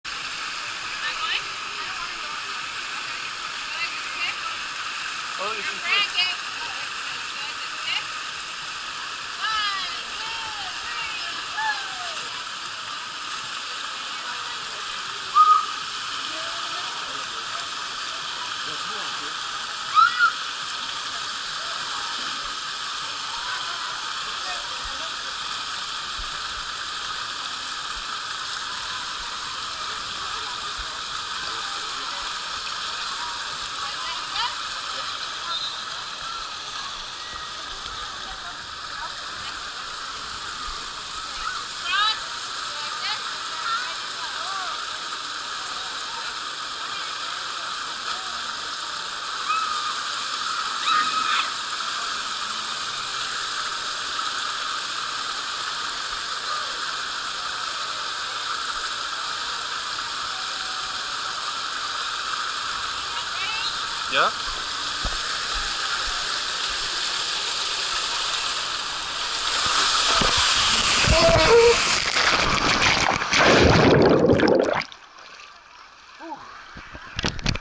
Bermuda Water Slide at Raging Waters Los Angeles
Bermuda water park slide at Raging Waters Los Angeles waterpark in San Dimas, California, United States. California’s Largest Water Park!